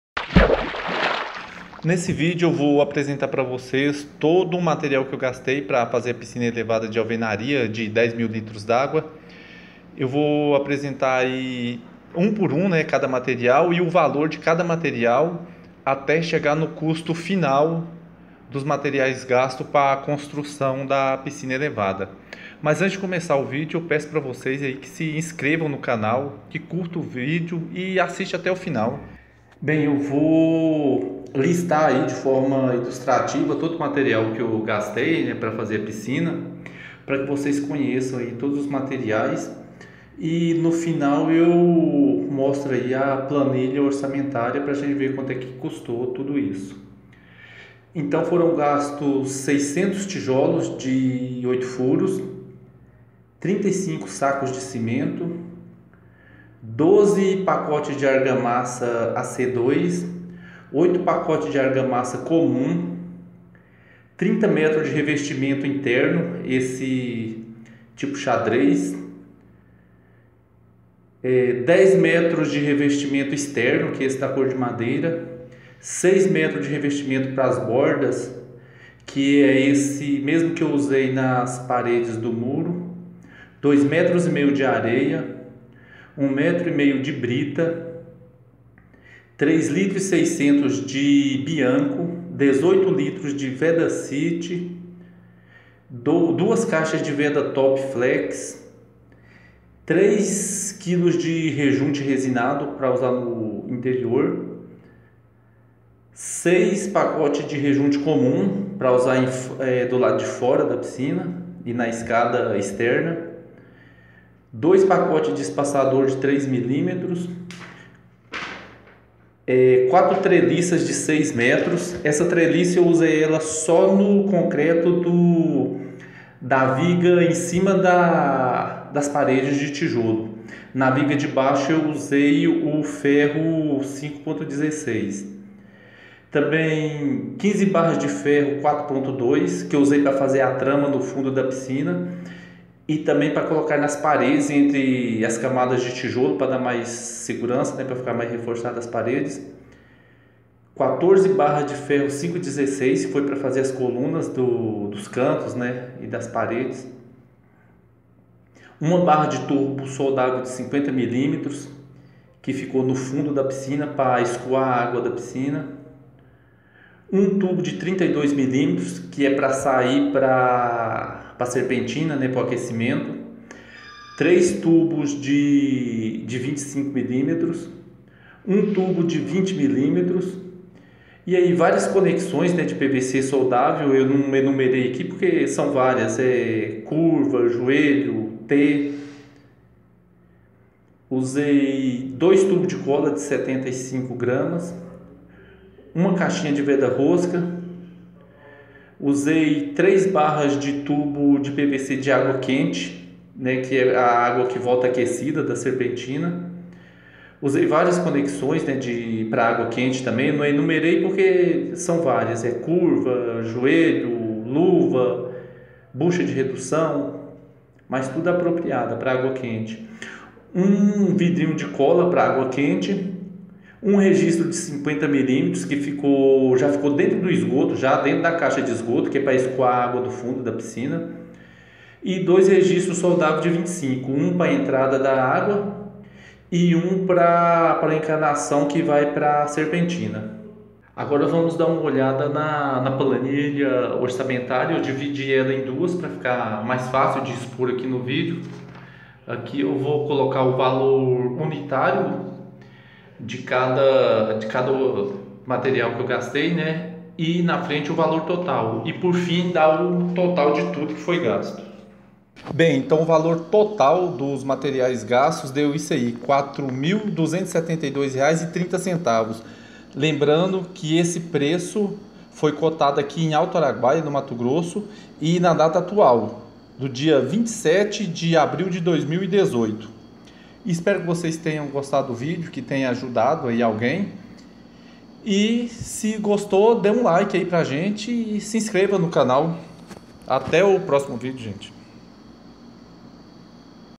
PISCINA ELEVADA DE ALVENARIA - QUANTO CUSTA PARA CONSTRUIR
LISTA COMPLETA DE TODOS OS MATERIAIS GASTOS PARA CONSTRUIR UMA PISCINA ELEVADA DE ALVENARIA, COM PLANILHA ORÇAMENTÁRIA.